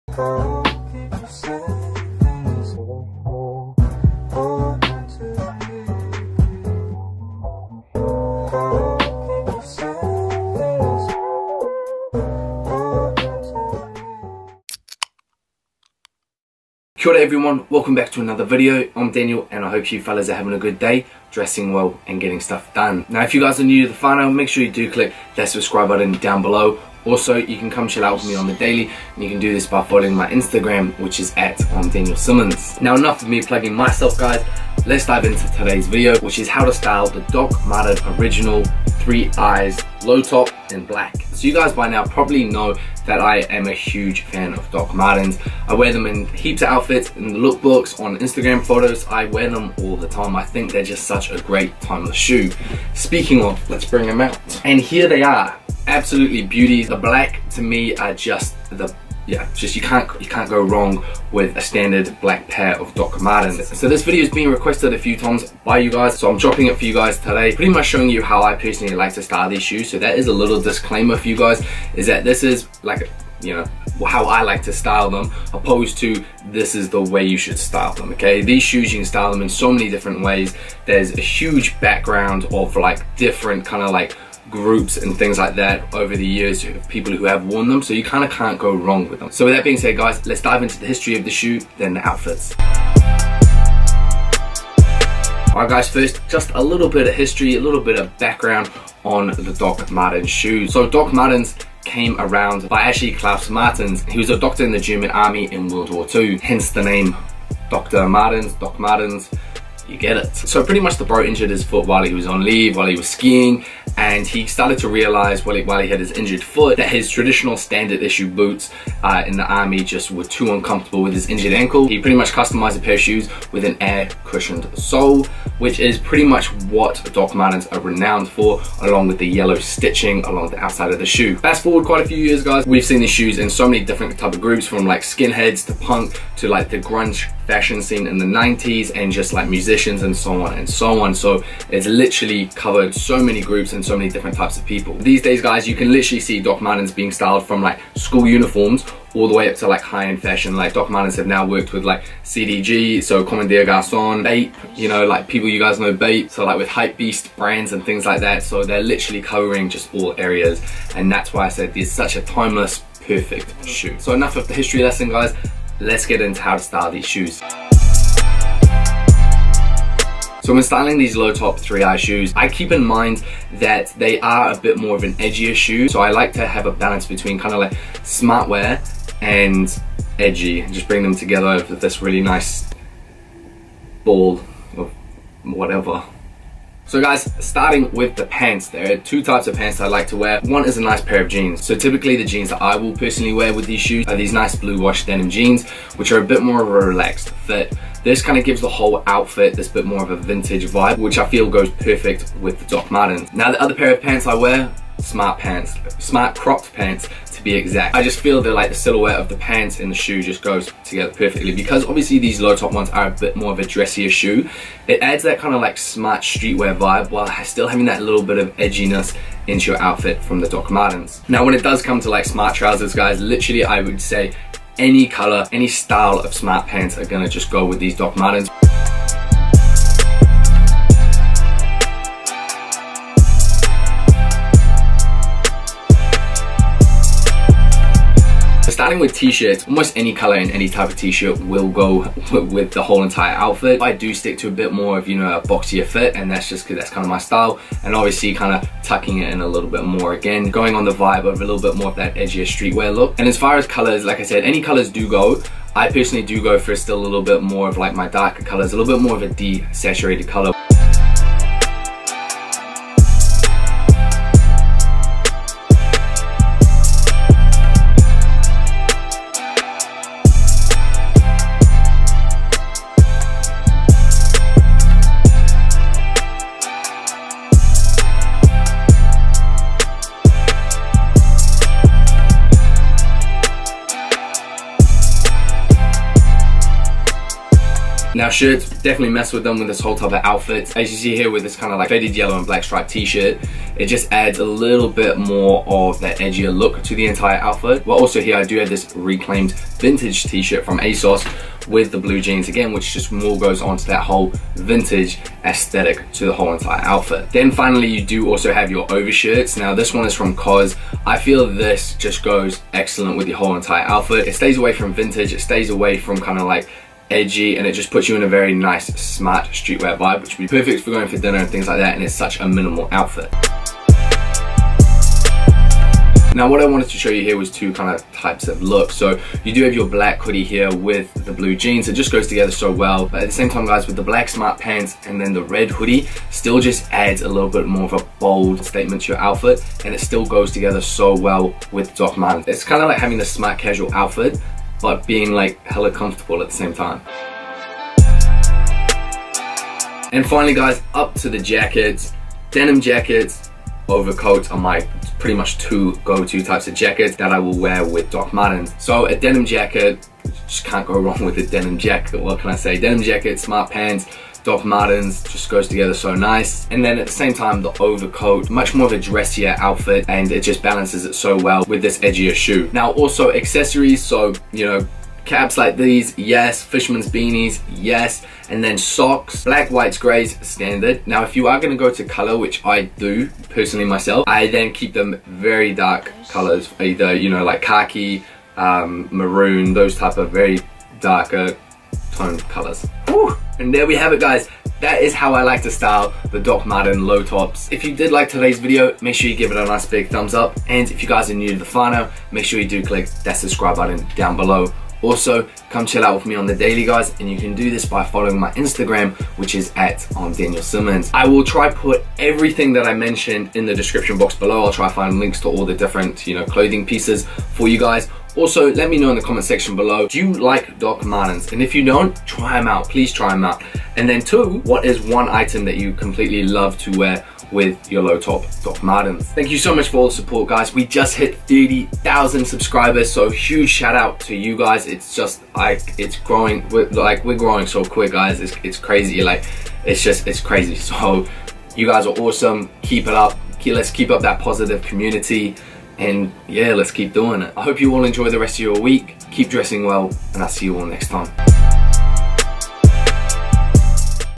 HOW TO STYLE DR. MARTENS 1461 | Men's Fashion | Lookbook | Daniel Simmons
Hey TEAM!
Today we are looking at 'how to style Dr.Martens 1461'. Ill be showing my the ways I like to style these timeless shoes!

+ Items in order

- Black Cropped Pants
- Grey Cropped Pants
- Brown Smart Pants

T-shirt:
- All Saints (sold Out) alternative
- White oversized
- Green T-shirt (alternative)

Shirt/Overshirt:
- Yellow/black stripes
- Oversized Vintage Shirt (alternative)
- COS Over-shirt (sold Out)

Hoodie:
- Black Hoodie
- Red Hoodie (alternative)

Jacket:
- Denim Jacket (alternative)

DEPOP: @dws713